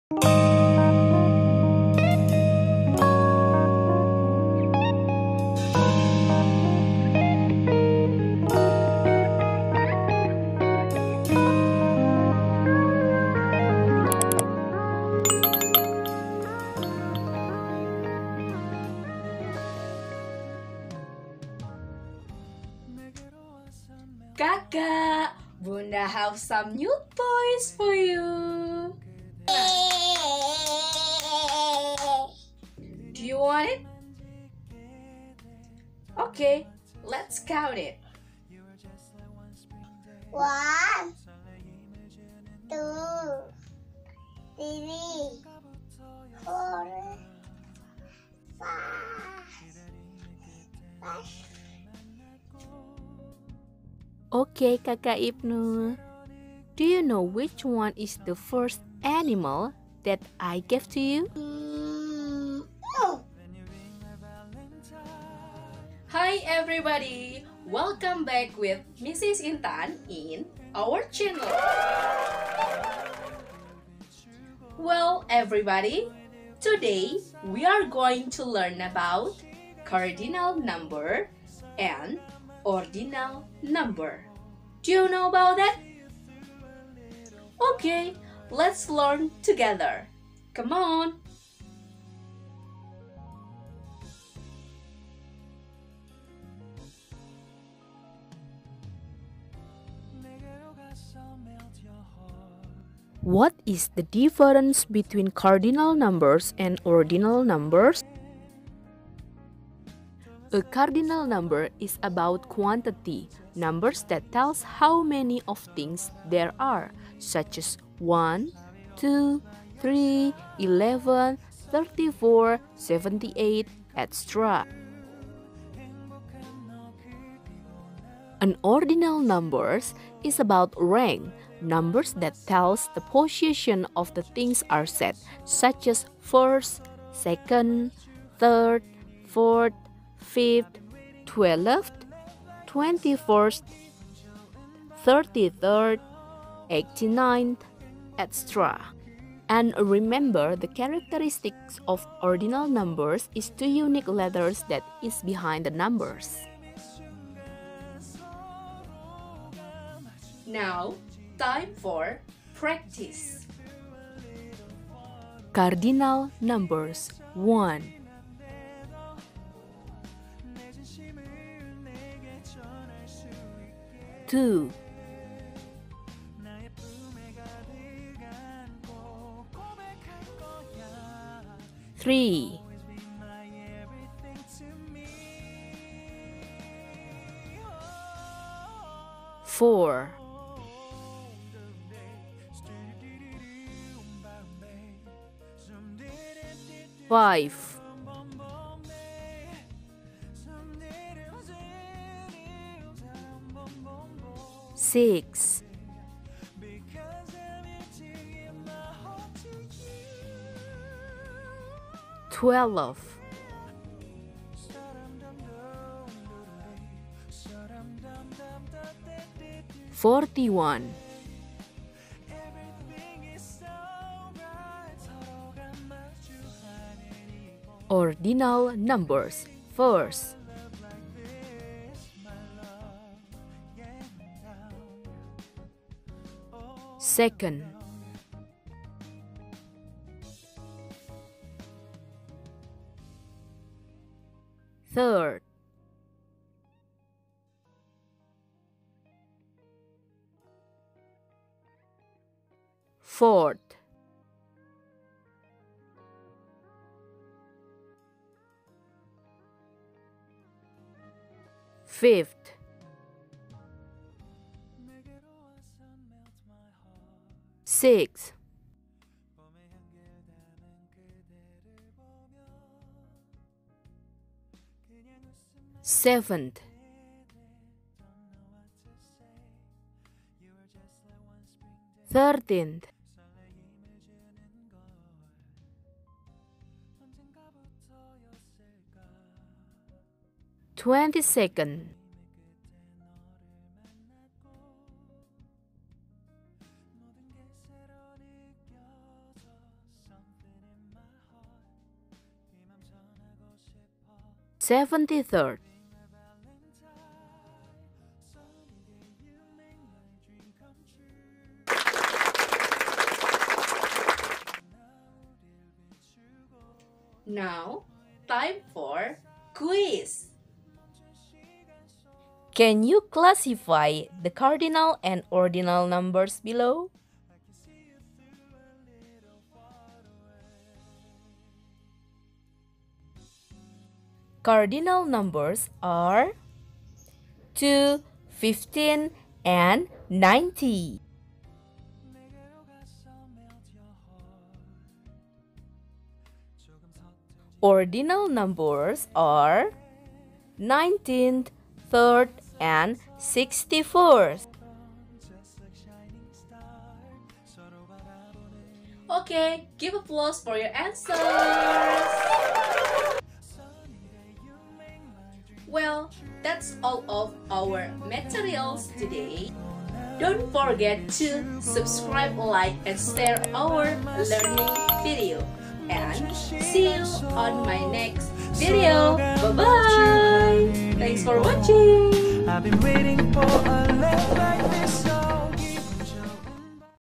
Cardinal and Ordinal Numbers
Materi Ordinal and Cardinal Numbers adalah materi sederhana yang terkadang sering kita lupakan. Materi ini dipelajari di pendidikan jenjang SD, SMP, dan SMA.

Mau request materi supaya dibuatkan? bisa banget, tinggalkan komen dibawah ini ya.